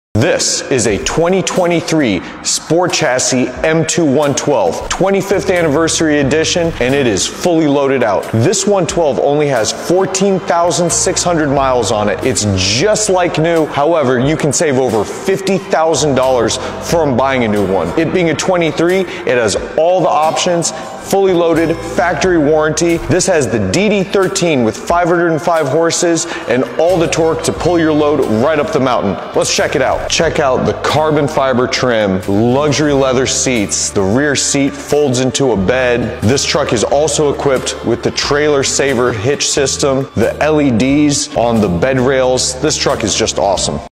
IT CAN DO WHAT??!
2023 FREIGHTLINER DD13 SPORTCHASSIS M2-112

ULTRA CLEAN BIG BLOCK
ONE OWNER TRUCK
ONLY 14K ORIGINAL VERIFIED MILES
505 HORSEPOWER
~1850 FT. LB OF TORQUE
TRUE FACTORY SPORTCHASSIS TRUCK
HUGE DD13 DIESEL MOTOR
6 SPEED 4000 SERIES ALLISON AUTOMATIC
HUGE 150+ GALLON FUEL CAPACITY